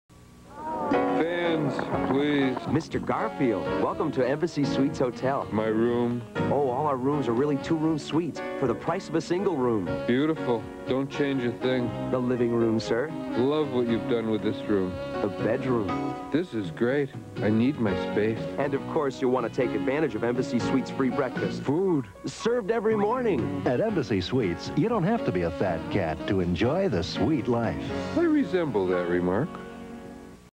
1986 Garfield Embassy Suites Commercial
Garfield takes a trip to Embassy Suites.

This was taken from a BetaMax tape of recorded television from around 1986.  Enjoy!